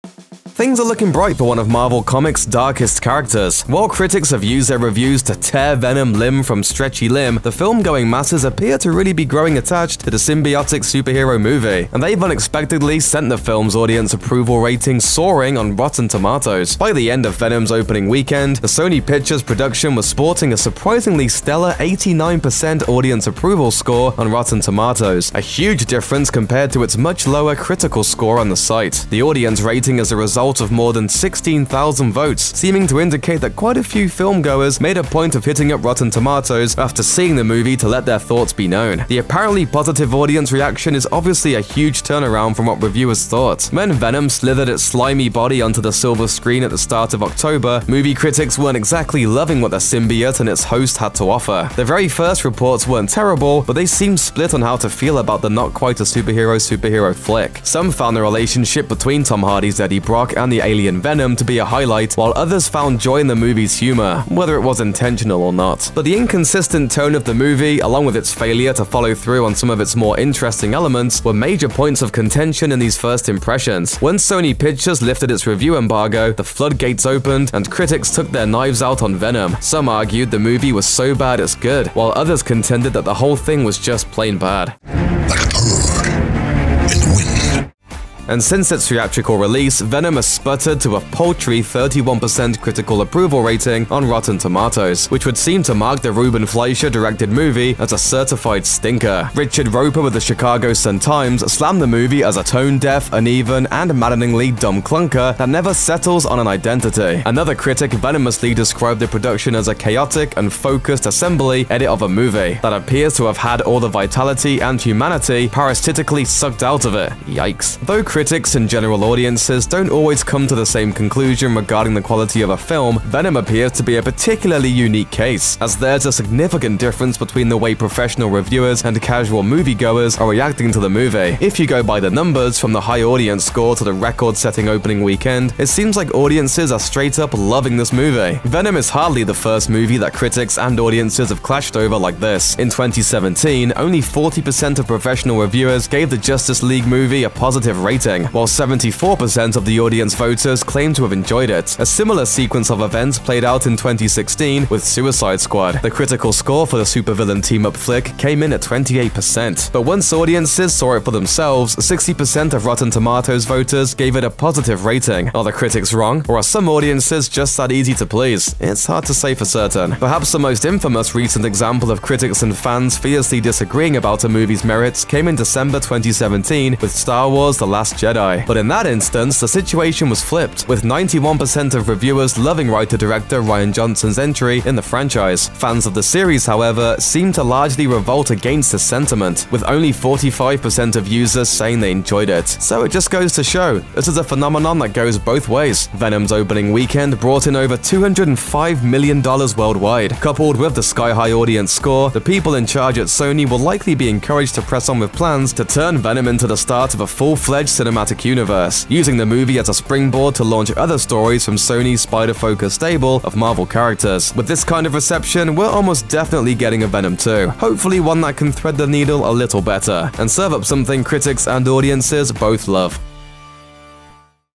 The Venom Audience Approval Rating Has Unexpectedly Soared
Tom Hardy’s Venom has finally made it to theaters, but critics have been far from kind to one of Marvel’s most iconic anti-heroes. Has the backlash from critics deterred fans away?

The answer is a resounding no. Venom is doing well at the box office, with fans filling seats, and even those who don't like the film have praised Hardy as Eddie Brock and Venom.

They are really loving what he is putting out, and they have unexpectedly sent the film’s audience approval rating soaring on Rotten Tomatoes. At the end of the day, you’ve got to give the audience what they want.